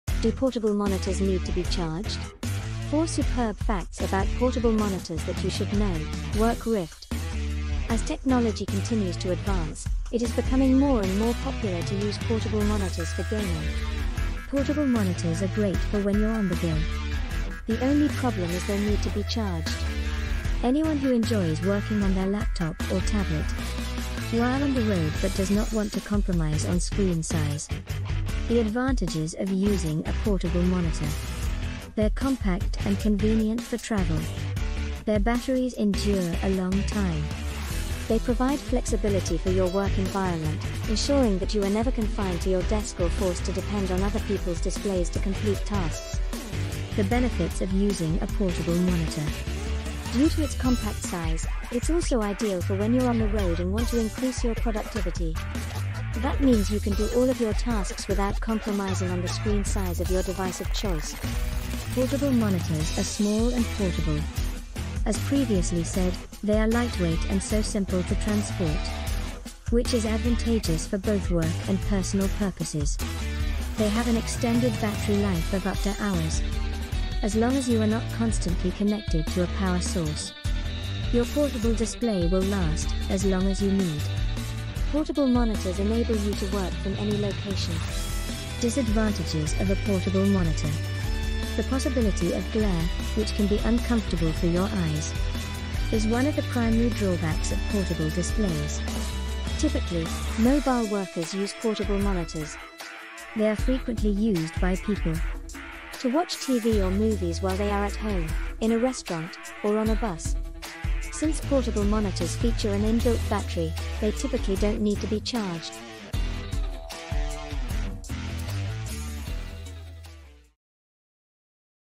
Do Portable Monitors Need To Be Charged? 4 Superb Facts About Portable Monitors That You Should Know
Portable monitors are great for when you’re on the go. They’re lightweight and don’t take up much space. The only problem is they need to be charged.
The battery life of these monitors is often not as long as a computer or laptop.

If you often use your laptop or tablet in multiple locations, such as an aircraft seat or a vehicle, you may also want a portable display.
It takes up little room and is simple to carry about due to its thin and lightweight design. Additionally, portable monitors are ideal for those who must operate in limited locations such as an aircraft seat or a vehicle.

To find out more regarding portable monitors, please read this article